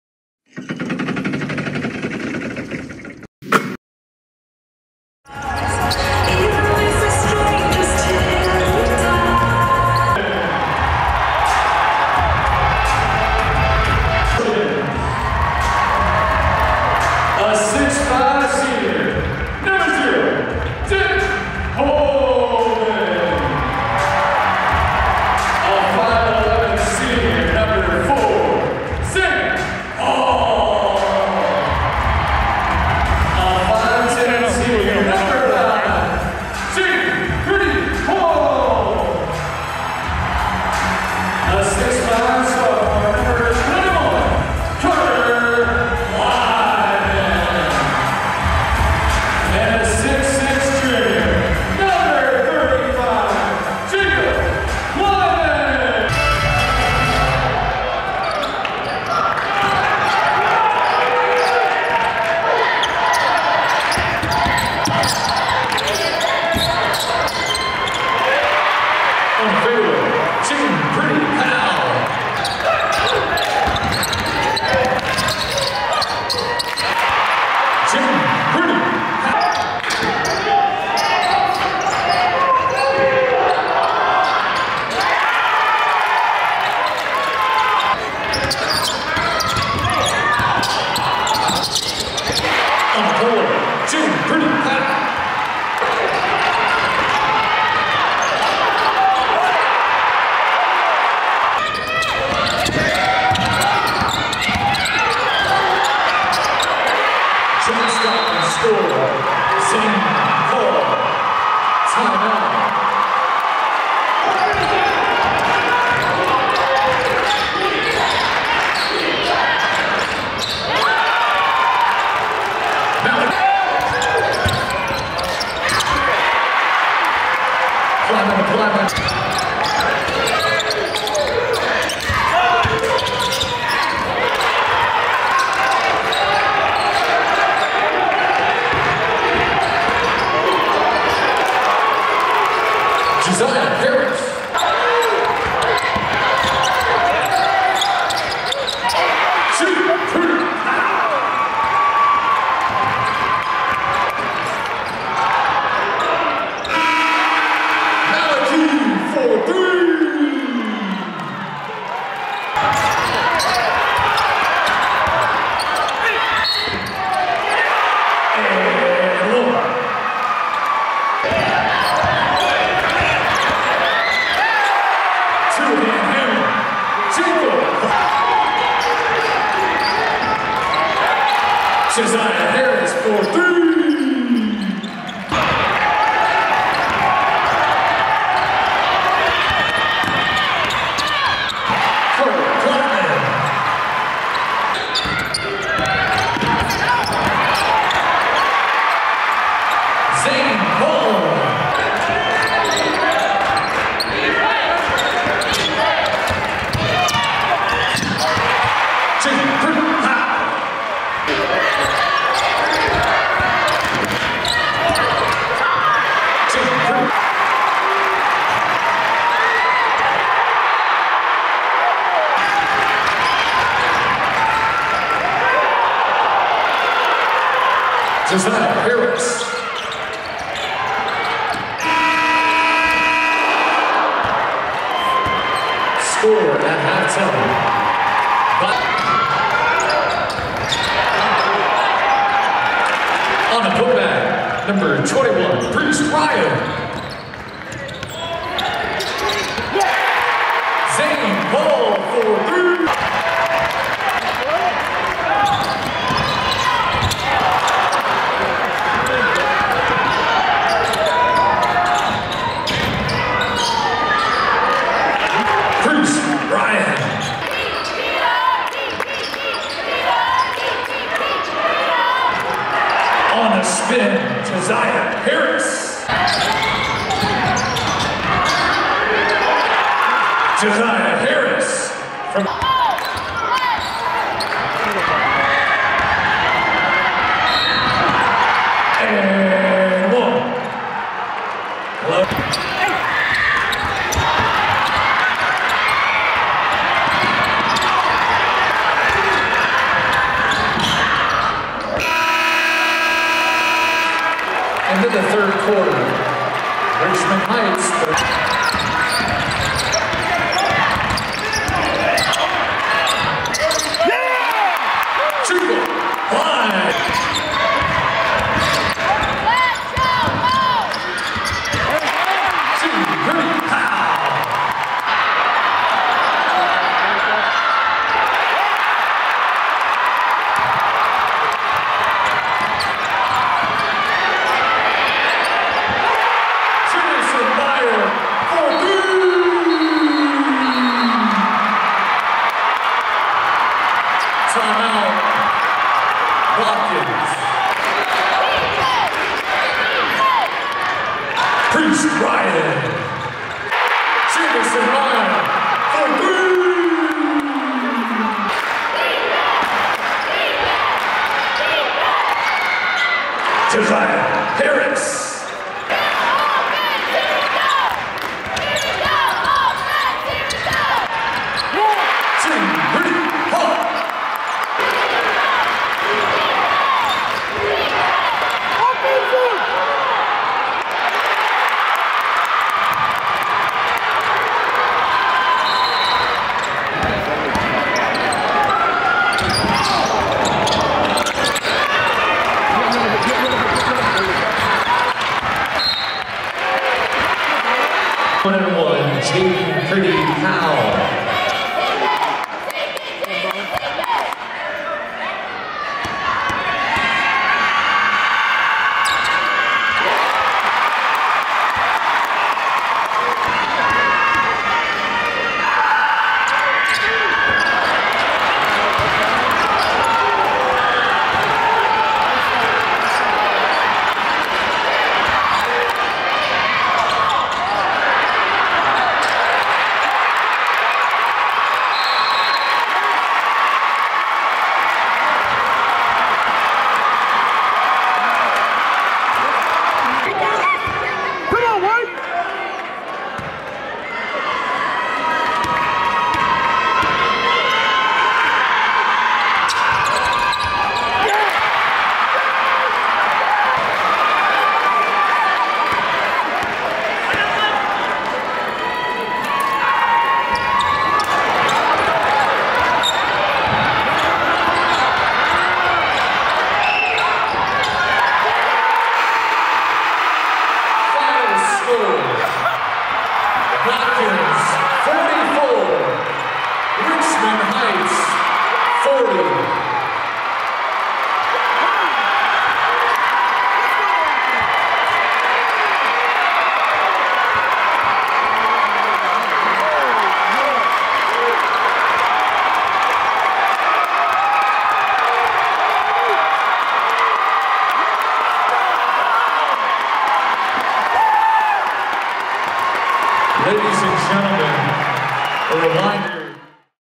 Botkins advances to its first ever State Title appearance! (Full Game Highlights)
A pair of late threes from Jameson Meyer and some gutsy plays from Jayden Priddy-Powell and Jacob Pleiman moved Botkins past Richmond Heights and to the Trojans' first-ever state final appearance. Late free throws from Jacob Pleiman sealed the victory and Botkins was led offensively by Priddy-Powell who paced the Trojans with 19 points.